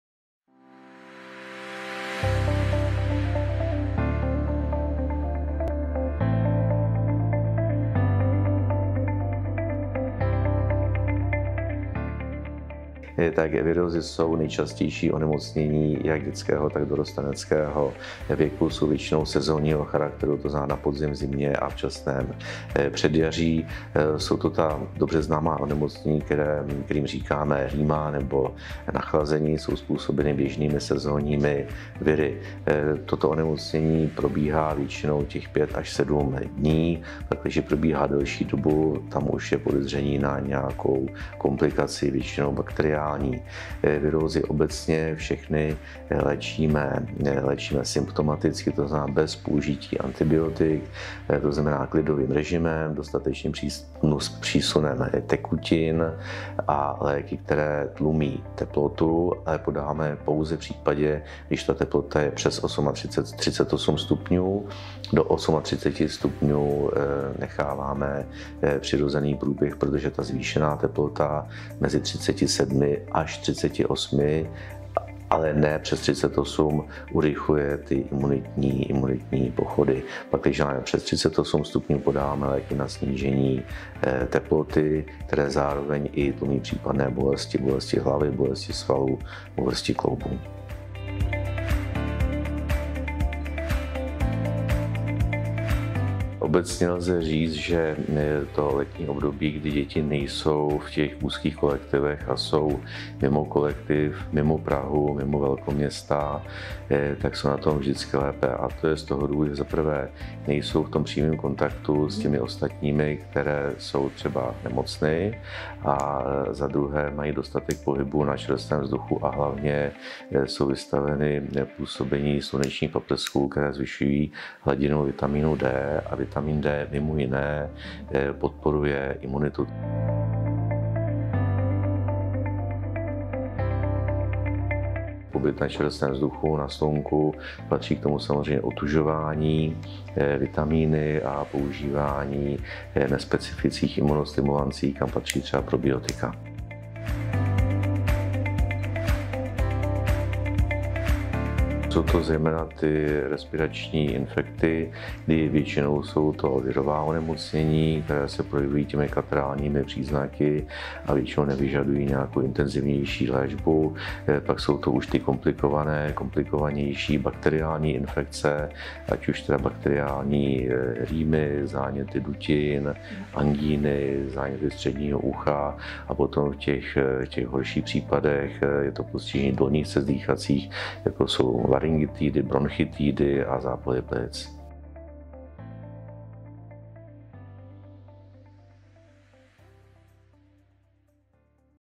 Návrat do škol: Imunitní systém u dětí
Pediatr MUDr. Petr Žáček vysvětluje jak podpořit imunitní systém u dětí.